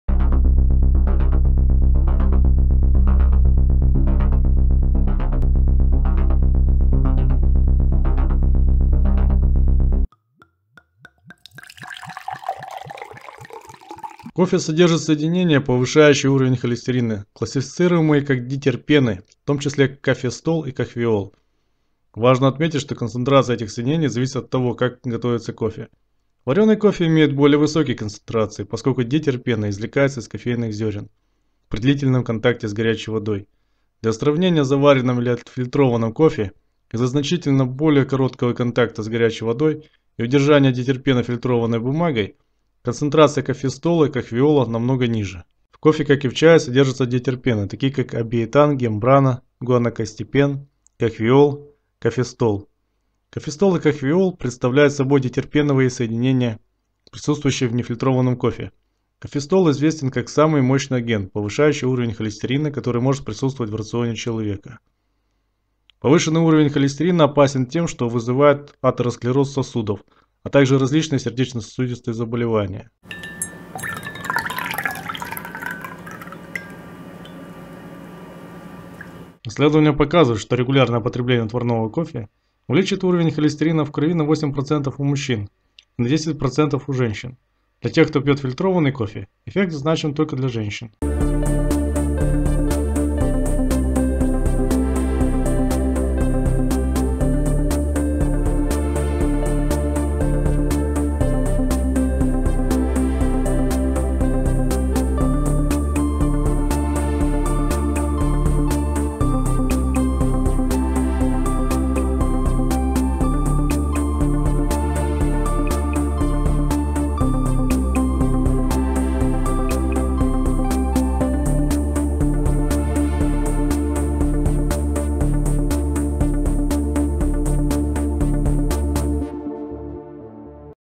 Кофе и дитерпены, повышеный холестерин
Многие сердечно-сосудистые заболевания связаны со стойким повышением холестерина, что говорит о нарушение жирового обмена и увеличеной скоростью инфильтрации сосудов. При длительной инфильтрации сосудов, особенно магистральных есть риск образования твердых бляшек с постоянным прогрессированием потологии. Кофе как один из факторов который влияет на рост бляшек в различных сосудах, начиная от аорты и заканчивая венами где зачастую еще является одним из факторов тромбоза вен, несет серьезную угрозу при постоянном его потреблении. Обжарка кофейных зерен кроме того, что дает различные концерогенные вещества, содержит в себе еще и дитерпены которые непосредственно учавствуют нарушении обмена веществ.